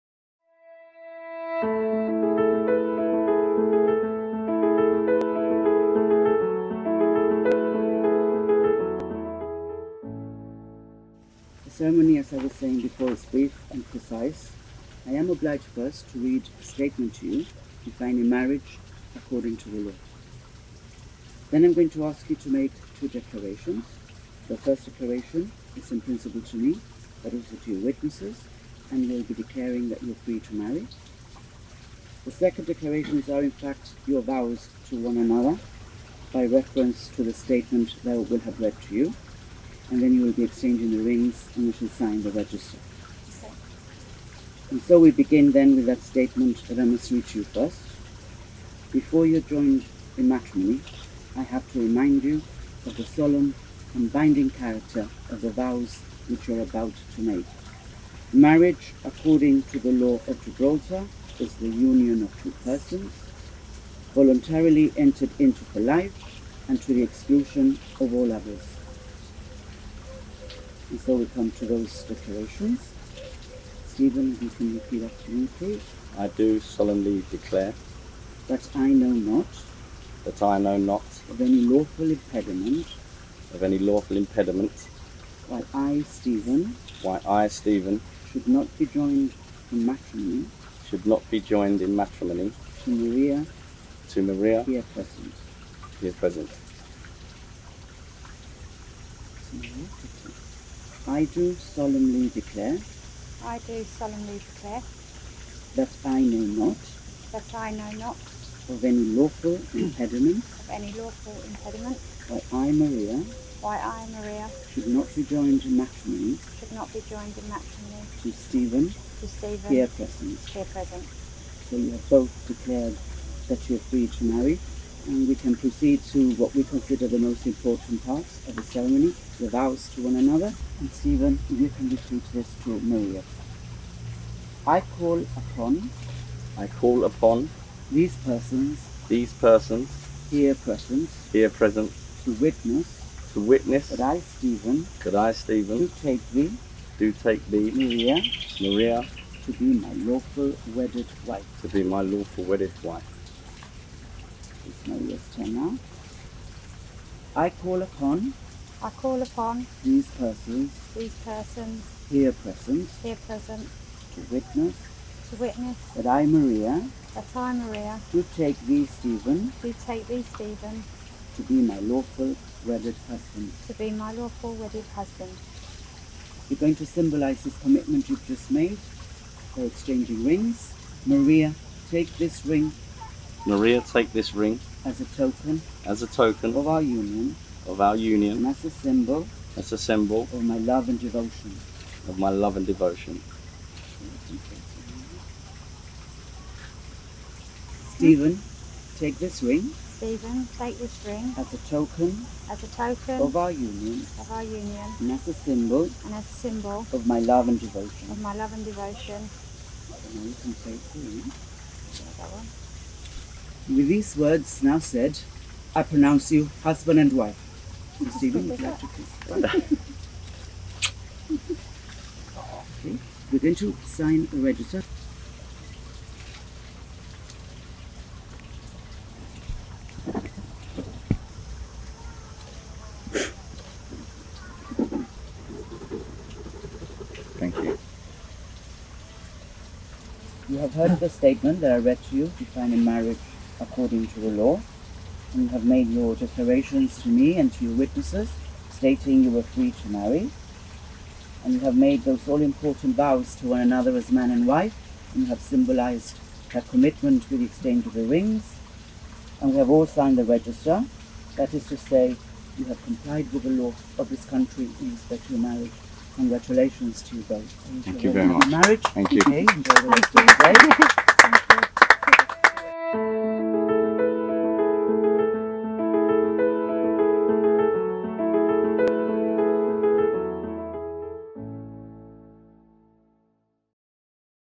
Maria & Steven’s Wedding
Maria & Steven’s Wedding, married on 17th August at the Botanical Gardens